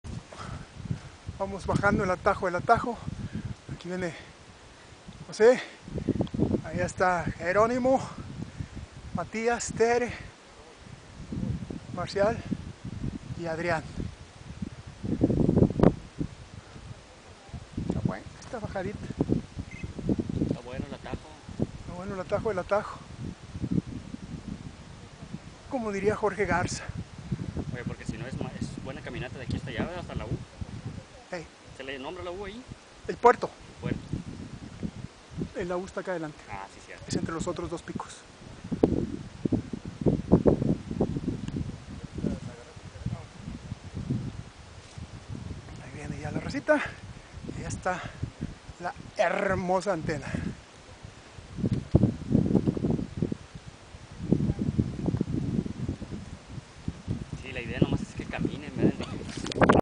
Cerro de la Silla, Antena-Pico Norte-Loma de la Virgen-Pico Sur-San Angel 7 de 30. Antena
"El Atajo del Atajo".
Por lo extenso del recorrido, hemos ido tratando de trazar "atajos" para abreviar el terreno y tiempo a recorrer.  Precisamente en donde se inicia el bosque de Encinos hicimos un atajo para entroncar con la ruta (insisto NO HAY VEREDA) que lleva del "Puerto del Oso" hasta la ladera de la Loma de la Vírgen de Guadalupe. . . No sabemos si el "Puerto del Oso" tiene es conocido con algún otro nombre, nosotros le llamanos así porque en una ocasión que ibamos cruzando Marcial Cerda, Agustín Leija y Yo de la Antena al Pico Norte, en ese punto nos salió un oso. . . Caminaba Yo adelante y a unos 10 metros me seguía Marcial y como a otros 15 metros de Marcial venía Agustín un poco rezagado.  Como a los 30 metros del puerto Marcial y Yo escuchamos como un susurro de Agustín diciendo "Un oso"; nosotros pensamos que Agustín estaba bromeando, pero dos veces más Agustín volvió a decir lo mismo y en la última ocasión lo hizo en voz alta y notablemente asustado. . . Marcial fue el primero que volteó y dijo: ¡es cierto es un oso!.  Yo saqué apresuradamente la cámara fotográfica, la puse en modo de video y, obviamente, el oso no me iba a esperar, para cuando traté de enfocar solo ví los arbustos de la ladera moverse agitadamente. . . a partir de ese momento y hasta acabar la excursión, Agustín ya no se separó de nosotros a más de 3 metros.